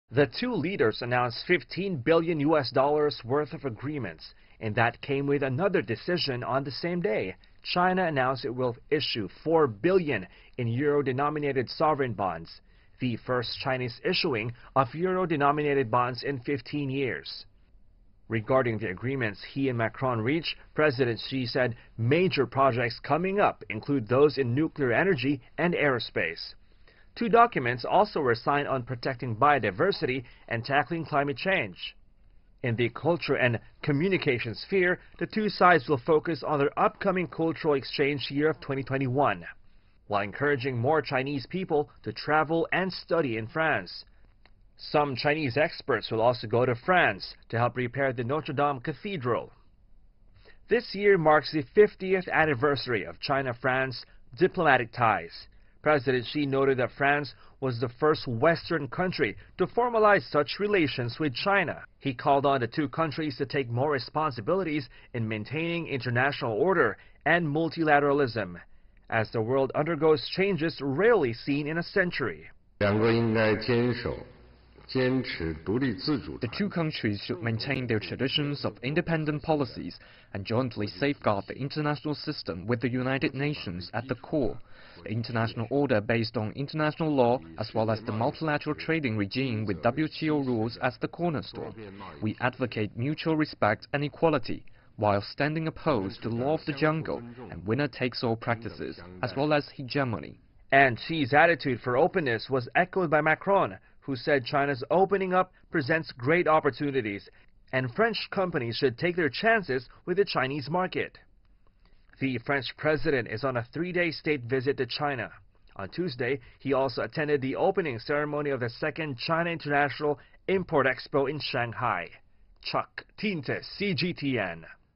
President Xi hosts French President Macron in Beijing
Chinese President Xi Jinping sat down with French President Emmanuel Macron. Their meeting led to billions of U.S. dollars worth of deals covering the environment, energy, and agriculture.